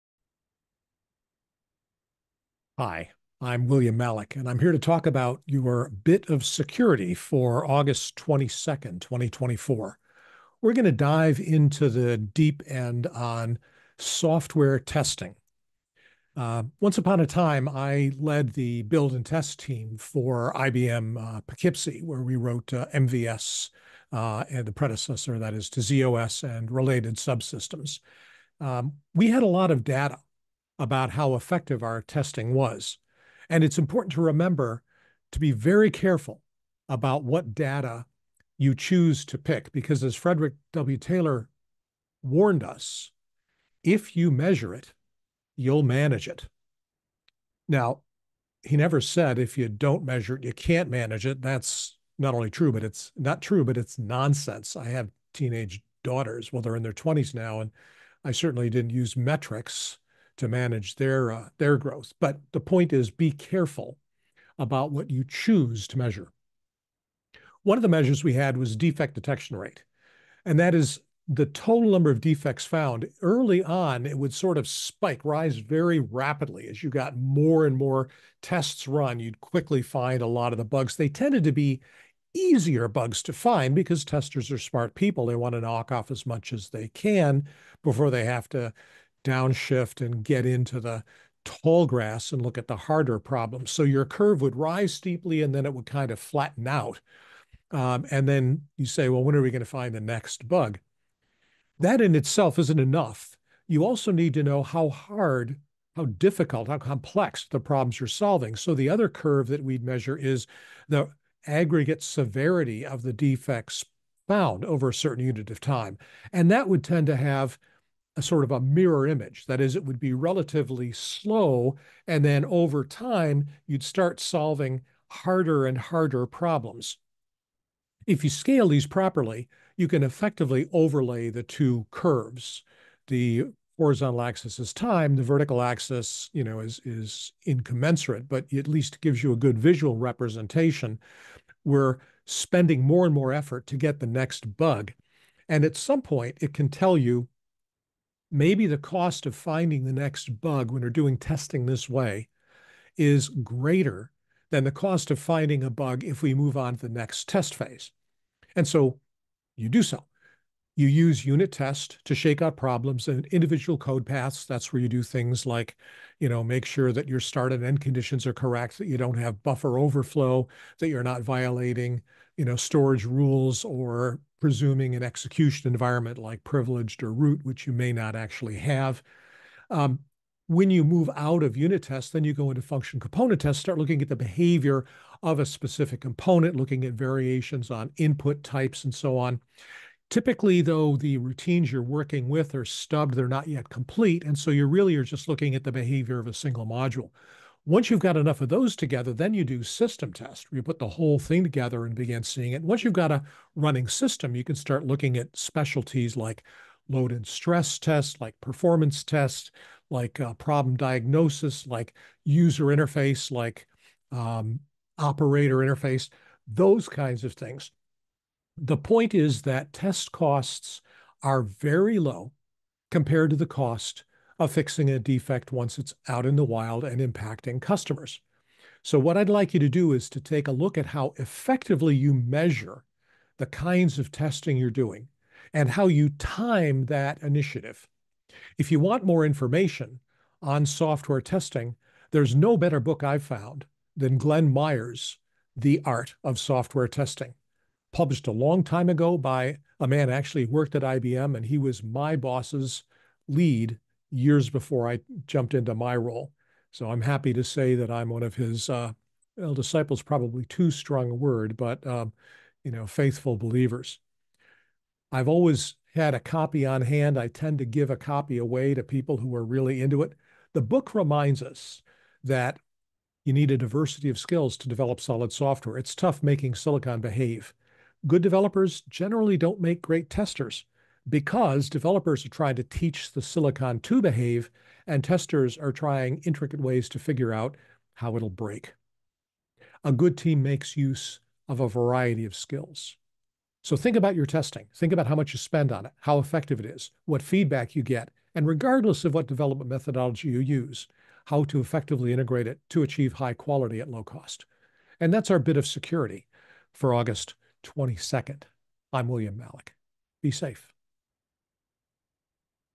Software Defect Detection
Defect Detection and Remediation Patterns E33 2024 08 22
When I guided the Build and Test team at IBM Poughkeepsie, we gathered plenty of data about defect detection and remediation rates. We used this data to track the effectiveness of our test efforts. Over time, we developed a pair of measures that did a good job predicting ultimate product quality and helped reinforce our understanding of the development and test schedule.
One measure was the defect detection rate. Typically, the total number of defects found would rise quickly at first, and over time it would taper off. Testers are efficient and knock off the easy bugs first. The second measure was the reported severity of the defects. That would tend to stay in the low to moderate range, and over time rise as the more difficult problems surfaced. Those would take more time and effort to root out. These two curves can be overlaid. One curve rises rapidly than asymptotically approaches a nearly flat slope, meaning there aren’t many more defects to find. The other curve stays low, and over time picks up.
At a certain point the cost of finding the next bug would exceed the cost of moving into the next test phase. Unit testing – evaluating code execution step by step – runs till those issues have been found and resolved. Input screening occurs during this phase – buffer overflow, privileged execution mode, and such should be validated very early. Then the testers move into function/component test, looking at subsystem characteristics, usually in a mocked-up environment, with stubs for incomplete routines. When those are available, at the right time, testing moves into system test – looking at whole system characteristics. With a running system we can begin to explore load and stress, recoverability, the user and operator interface, input and environmental variations, installation and packaging, and failure modes.
The point is that test costs are very low compared with fixing defects in production, so staging your test initiatives to optimize your test efforts can save you a lot of heartache and long evenings downstream.
For more insight into testing, see “The Art of Software Testing” by Glenford Myers. I’ve given away more than a few copies, but I always have one on hand. The book reminds us that good developers generally make lousy testers – and how a good team makes the best use of all types of skills.
Defect Detection and Remediation Patterns
A Bit of Security for August 22, 2024
How do you measure the effectiveness of your testing? What is worth counting, and why?  Listen to this -